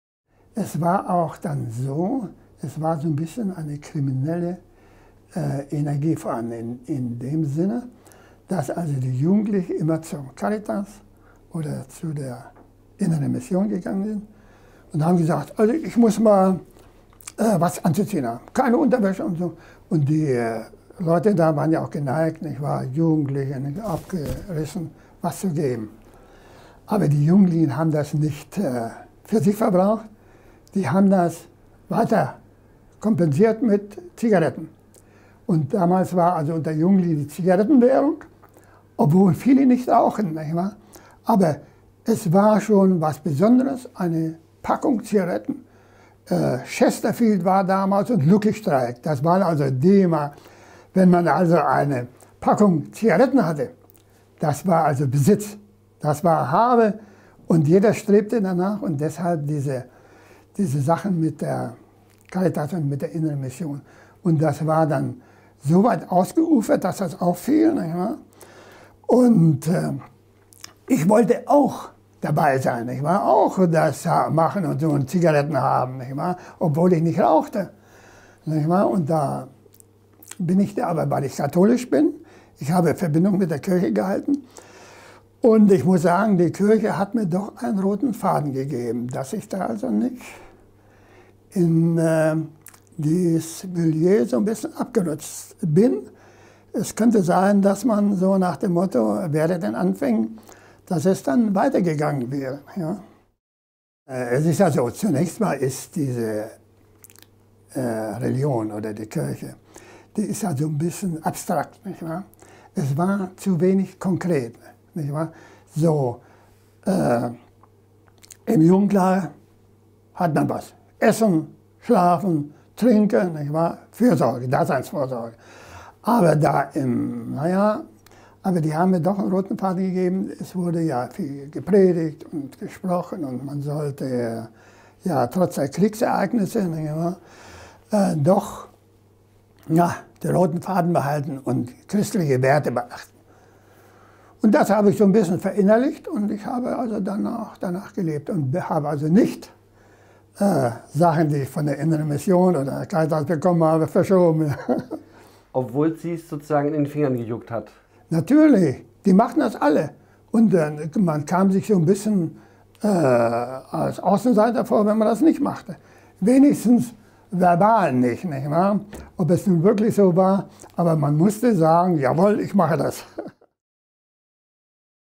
Gerhard Kolbe: Versuchung und Glaube
Gerhard Kolbe flüchtete 1947 nach Westdeutschland und kam in der evangelischen Auffangstelle für Jugendliche in Friedland unter. Der damals 16-Jährige erzählt von den Fehltritten der jungen Männer und wie die Kirche ihnen helfen wollte.